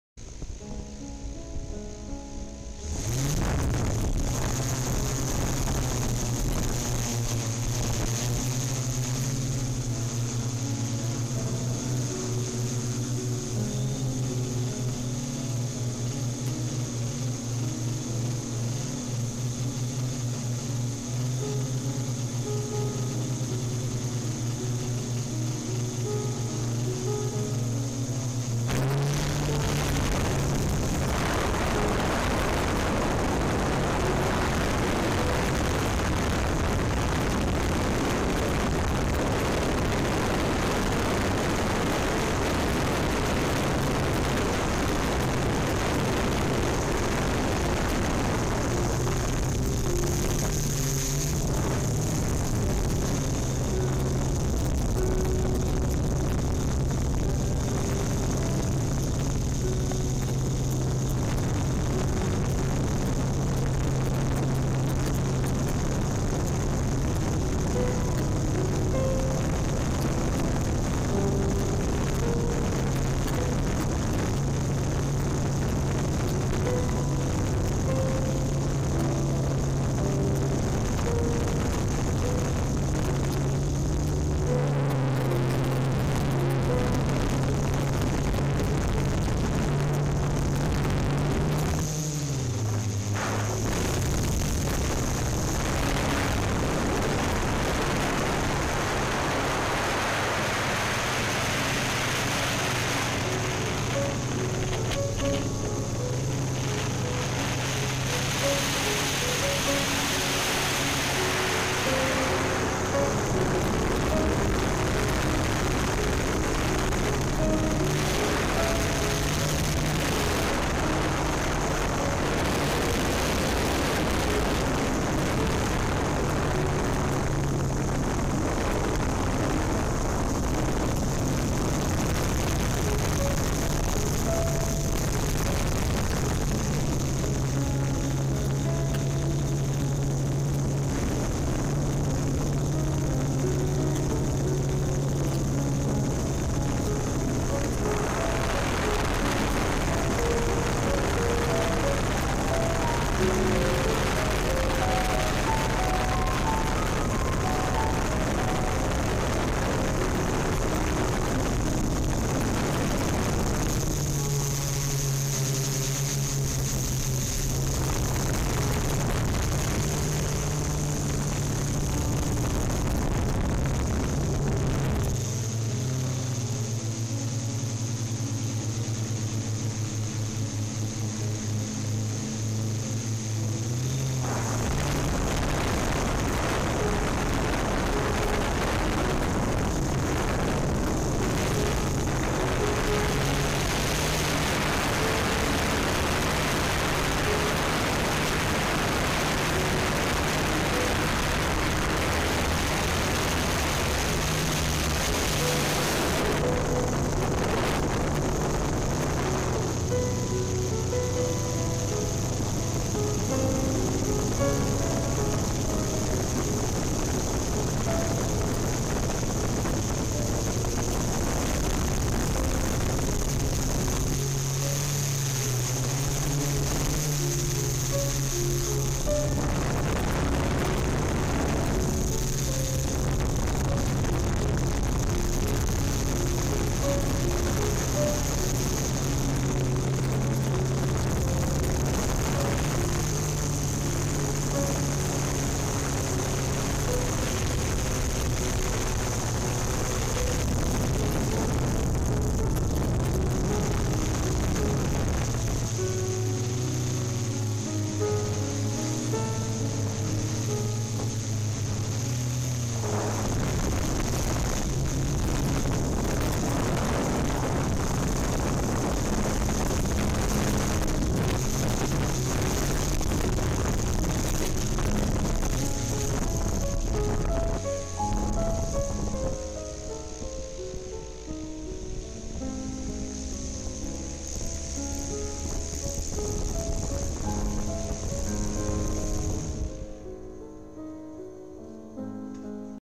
Playa de puerto Marqués, Acapulco México
Éste video fue tomado en la playa de puerto Marqués, Acapulco México con el drone yunecc typhoon Q500+ y con la cámara cgo2+,Gracias por mirar éste video.